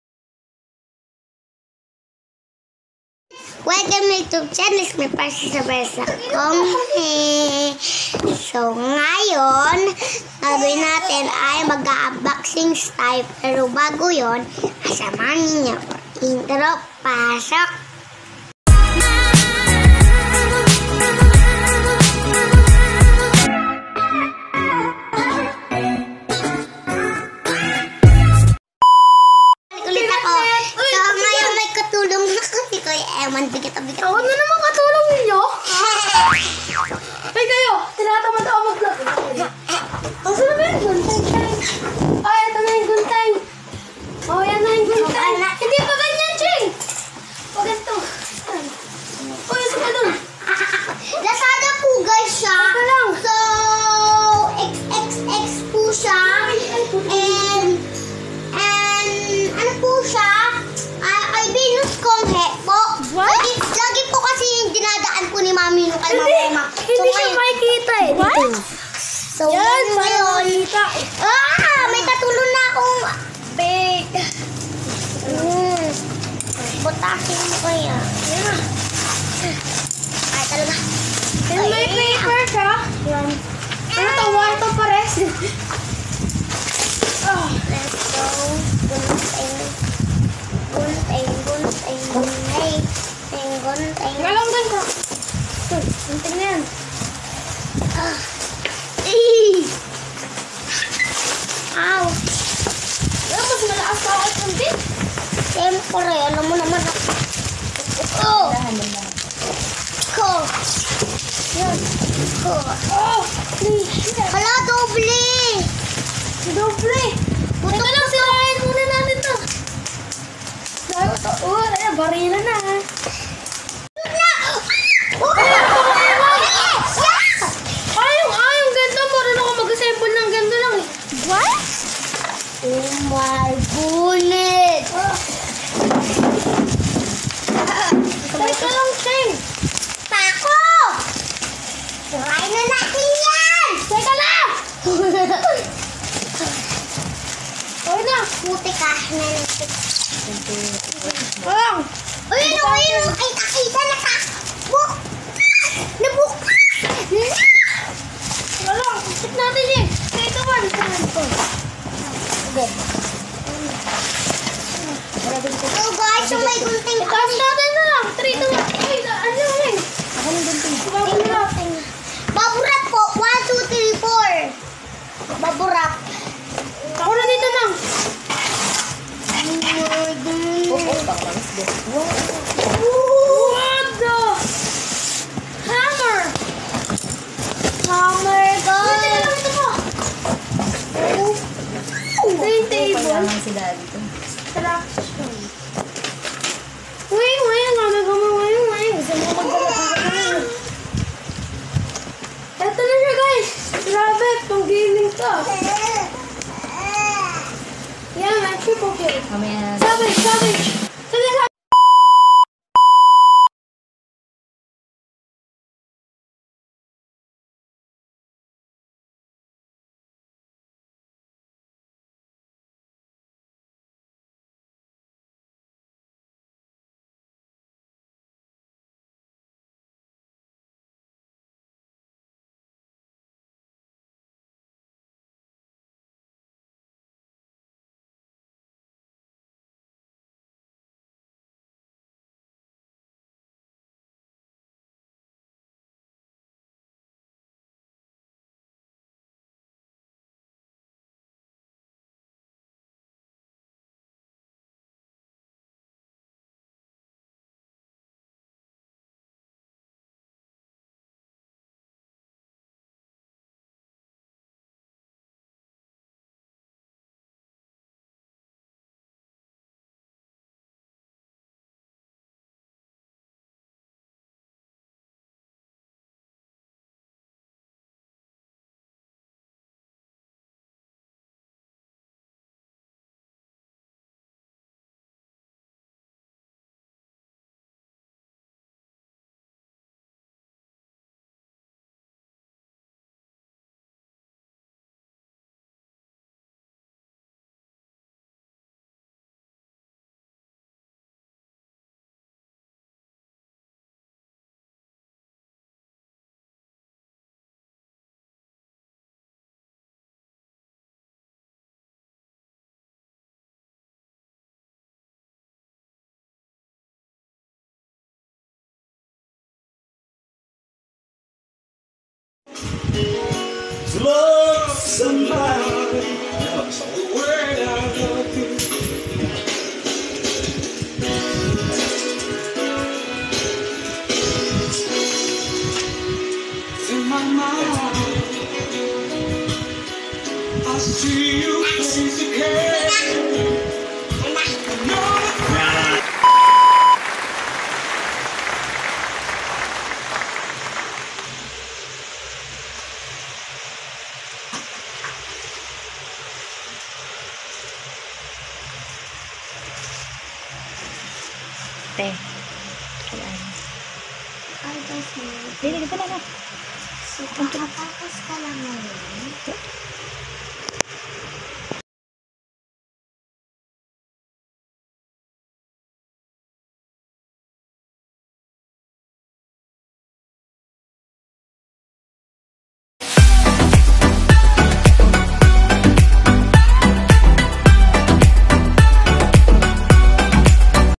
New Computer Table | For my Online Class | Francine Ysabelle | Vlog#10
My Mommy bought a table for my everyday online class and very comfy and worth it to use thank you Mommy and also thank you daddy for  fixing it.Keep safe everyone..just follow .like and subscribe Godbless Everyone!